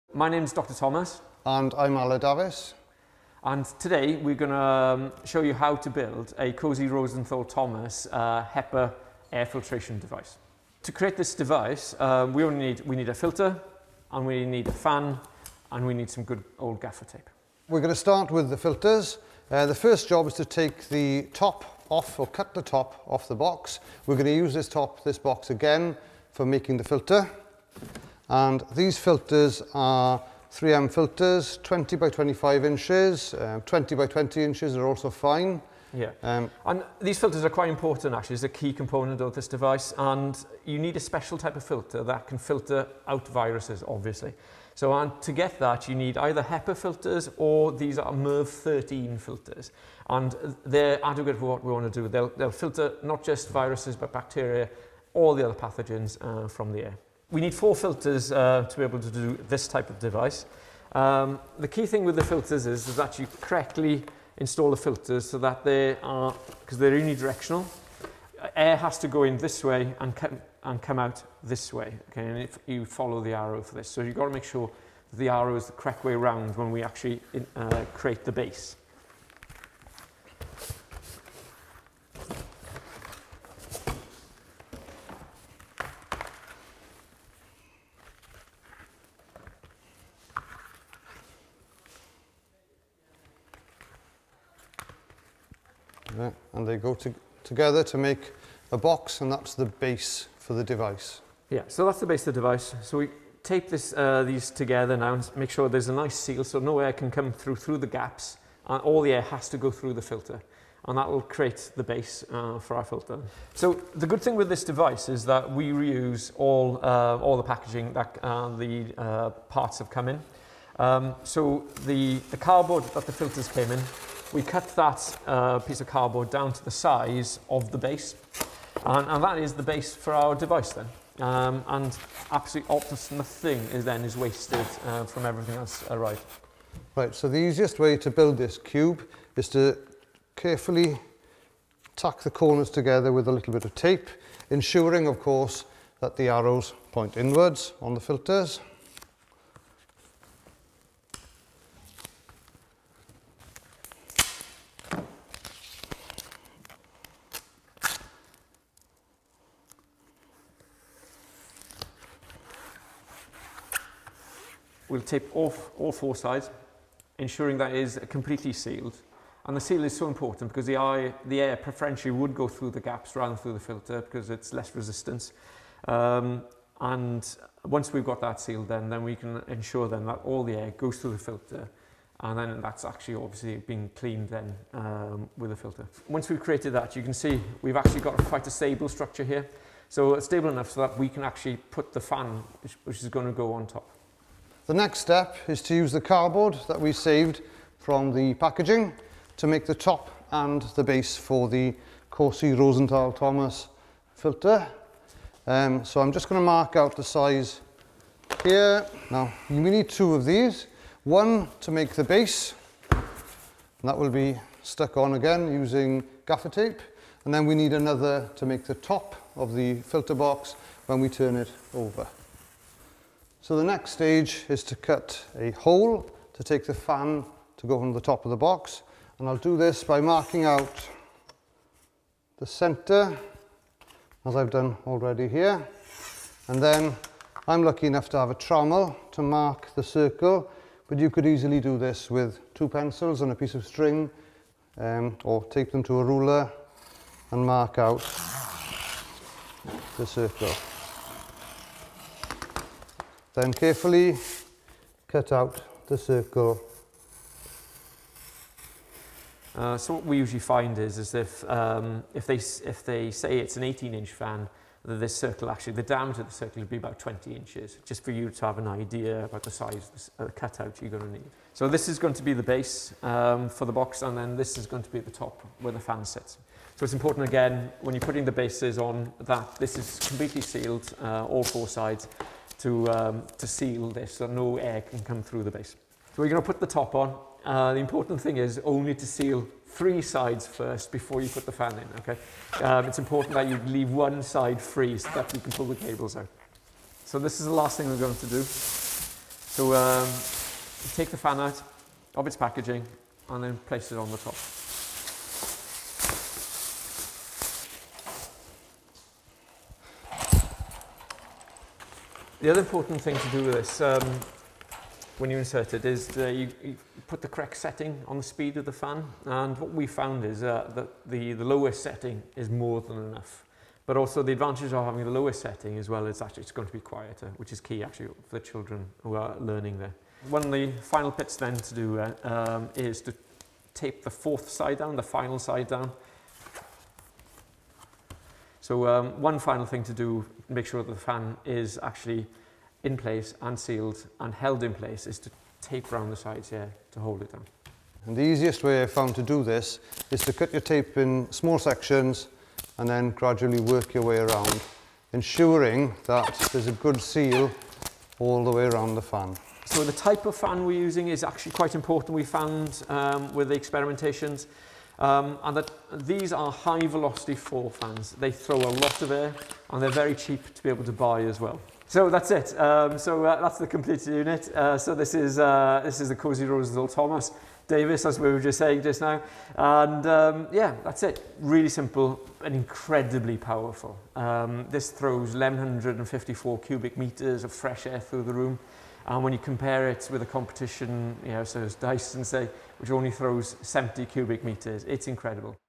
DIY step by step Build Video Corsi Rosenthal Thomas CR Box Wales Cymru
Dr Rhys Thomas demonstrates how to build a Corsi Rosenthal Thomas CR Box to assist with the highly effective ventilation of classrooms and other spaces

Parts Used ...

Reference ...

CTEX ...